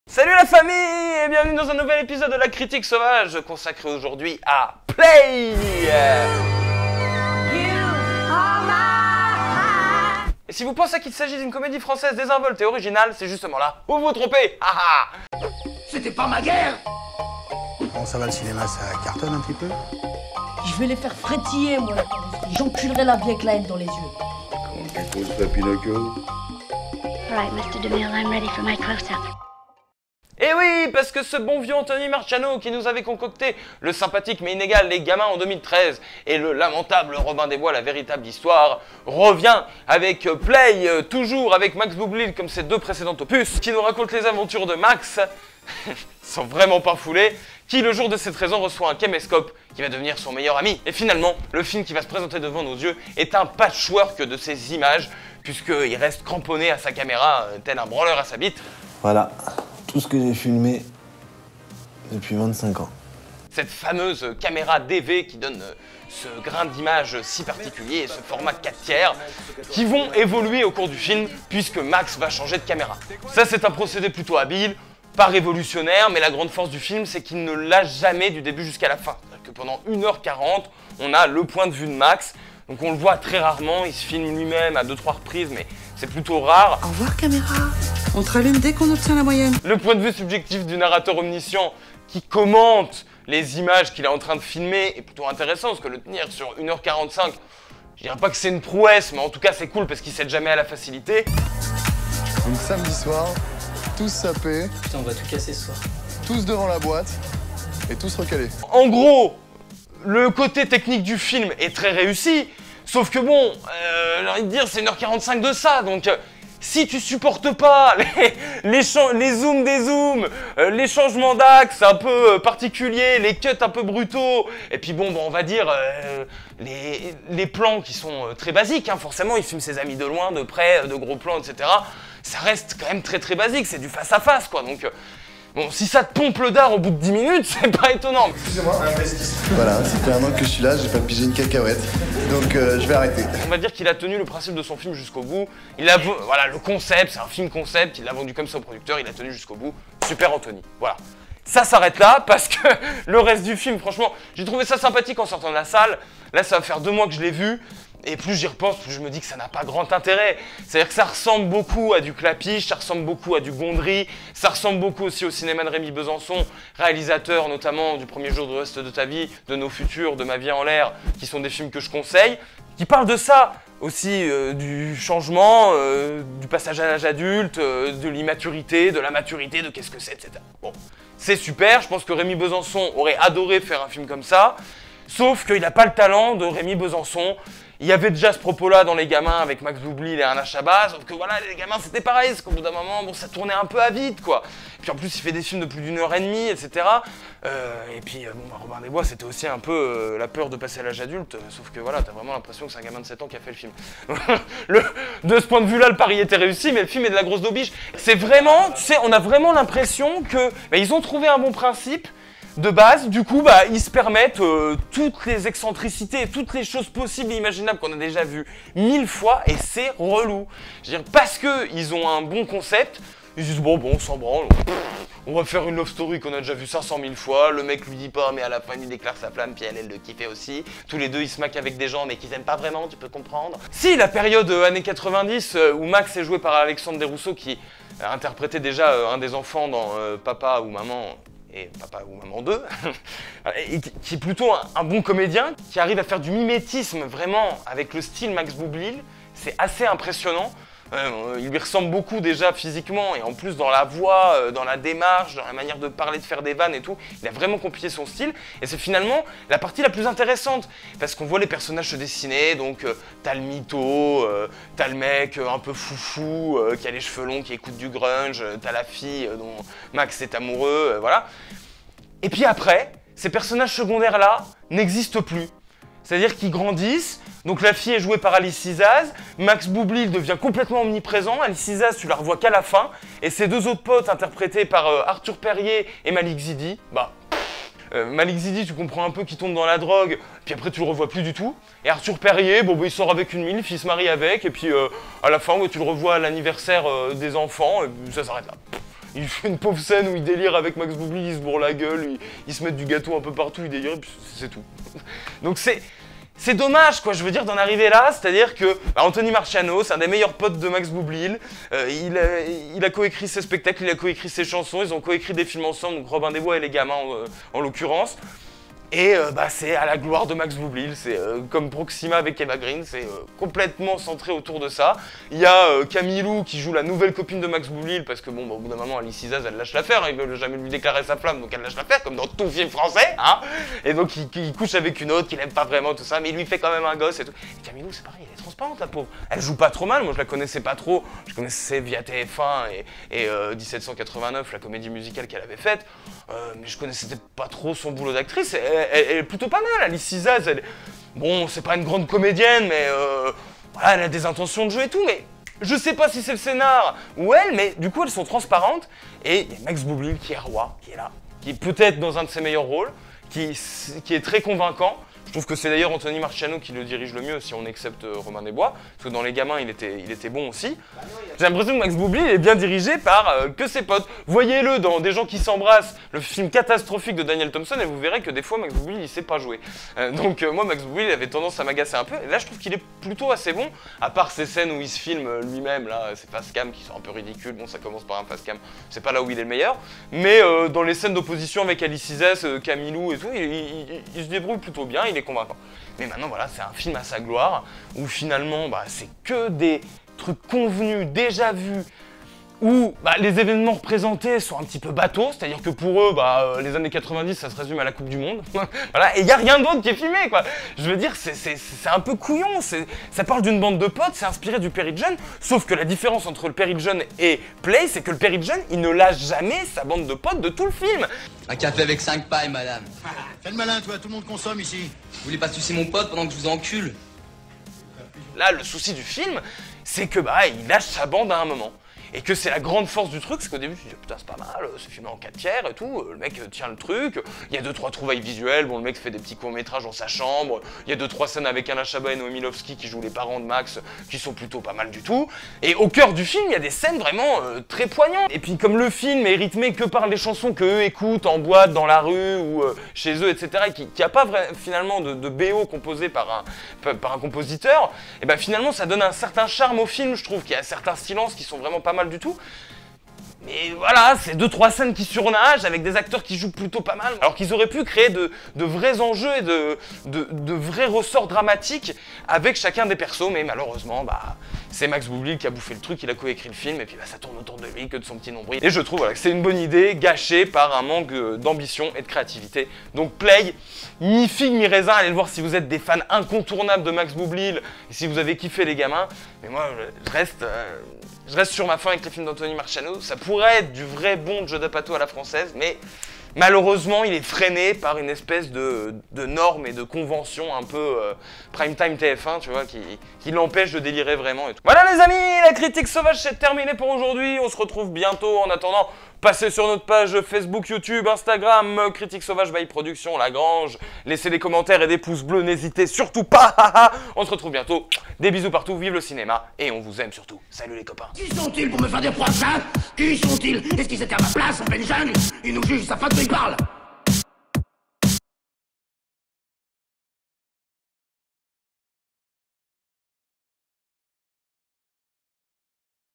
Play - Critique
Critique filmée consacrée au film Play  réalisé par Anthony Marciano, sorti depuis le 1er Janvier 2020 et distribué par Gaumont.